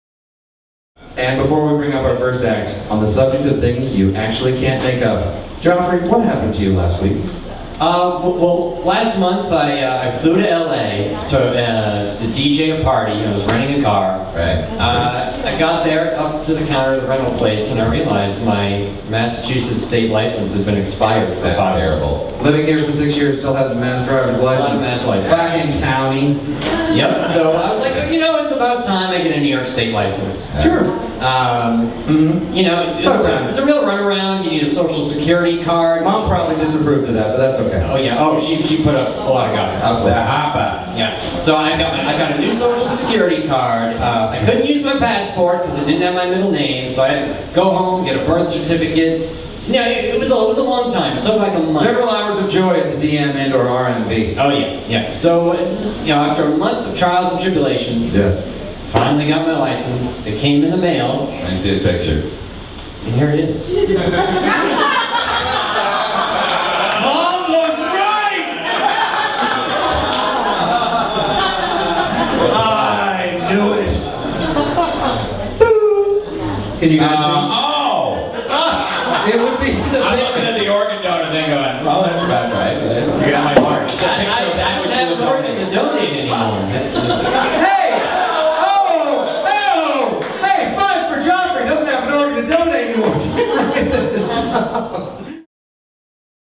Geoffrey's New License
Looks like someone at the New York State DMV made a small data entry error...